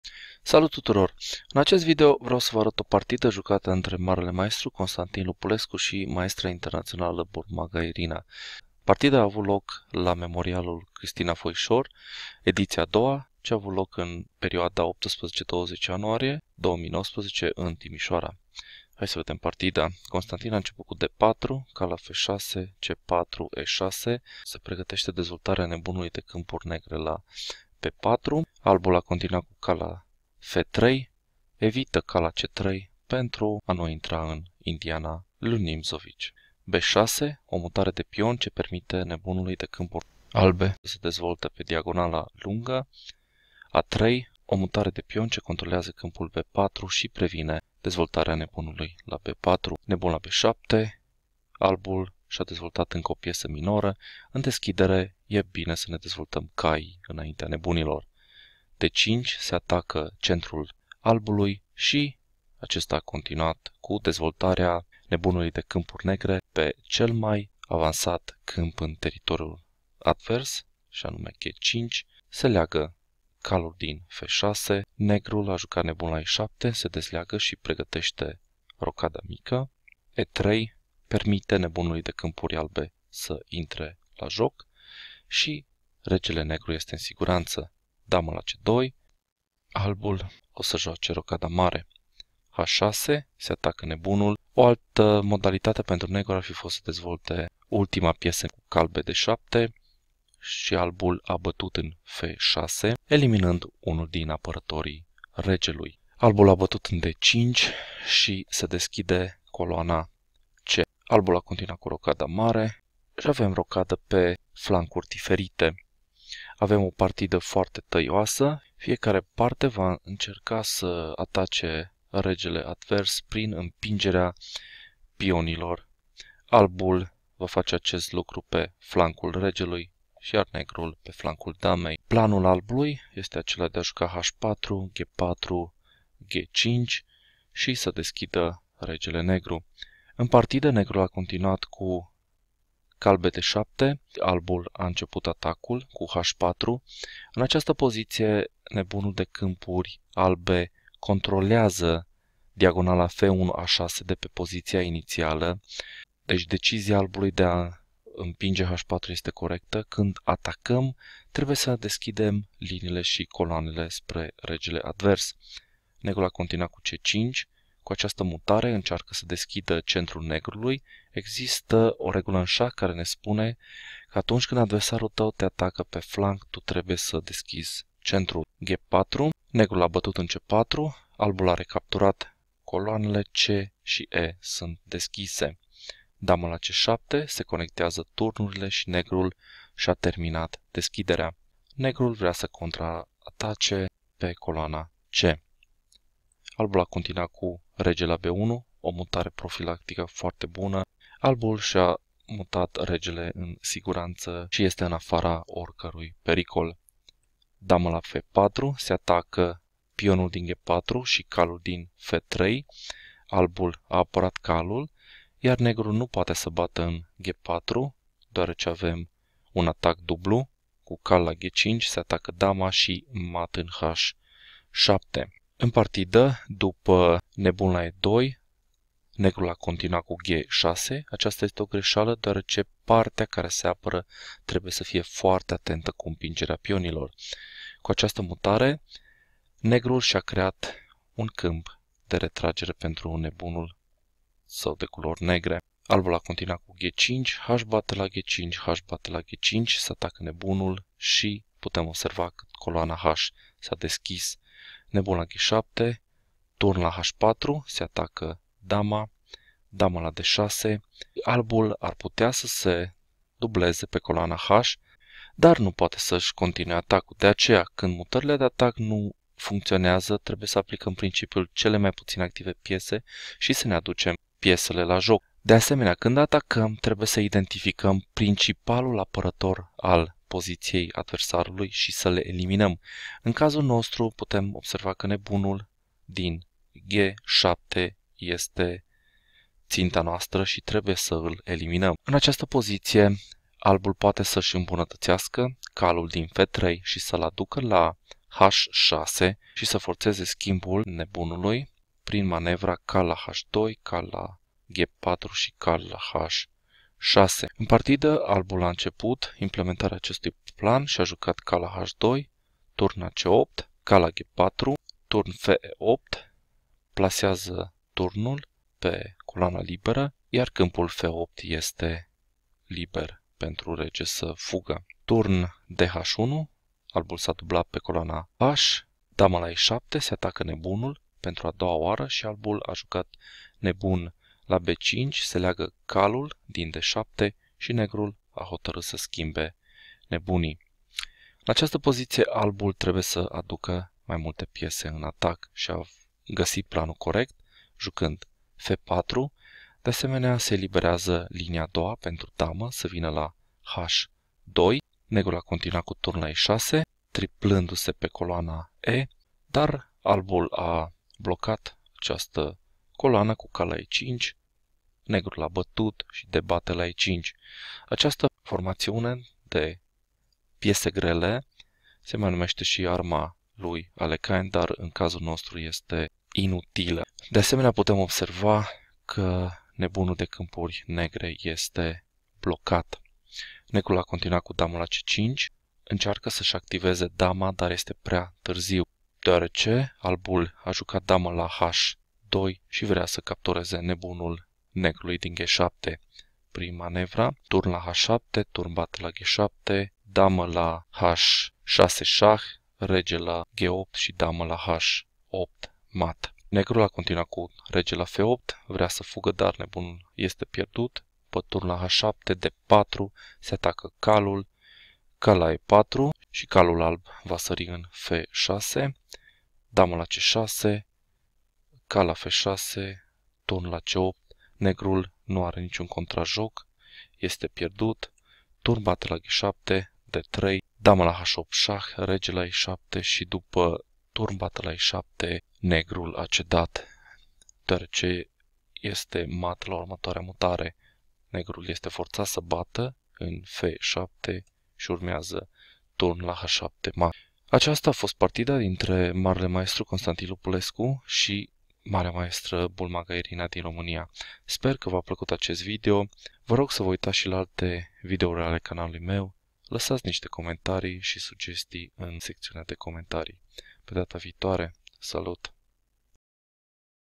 [Memorialul Cristina Foisor] Constantin Lupulescu vs Bulmaga Irina R4 Indiana damei [Romanian ver]
In this video vreau sa va arat o partida jucata intre GM Constantin Lupulescu si IM Bulmaga Irina. Partida s-a jucat la memorialul Cristina Foisor editia a 2-a ce a avut loc intre 18-20 ianuarie 2019 in Timișoara.

[Event "Cristina Foisor Memorial 2nd edition"]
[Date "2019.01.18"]
[Round "4"]
[White "Lupulescu Constantin"]
[Black "Bulmaga Irina"]
[Result "1-0"]
[Variant "Standard"]
[ECO "E12"]
[Opening "Queen's Indian Defense: Kasparov-Petrosian Variation, Main Line"]

1. d4 Nf6 2. c4 e6 3. Nf3 b6 4. a3 Bb7 5. Nc3 d5 6. Bg5 Be7 7. e3 O-O 8. Qc2 h6 9. Bxf6 Bxf6 10. cxd5 exd5 11. O-O-O Nd7 12. h4 c5 13. g4 cxd4 14. exd4 Qc7 15. Kb1 Qf4 16. Be2 g6 17. g5 hxg5 18. hxg5 Bg7 19. Rh4 Qd6 20. Nh2 Rac8 21. Ng4 Rfe8 22. Rdh1 Qe7 23. Bb5 Bc6 24. Bxc6 Rxc6 25. f4 Re6 26. Ne5 Nxe5 27. dxe5 Qc5 28. Qh2 Kf8 29. Rh7 d4 30. Ne4 Qc6 31. Nf6 Rc8 32. Rxg7 d3 33. Qh8+ Ke7 34. Rxf7+ 1-0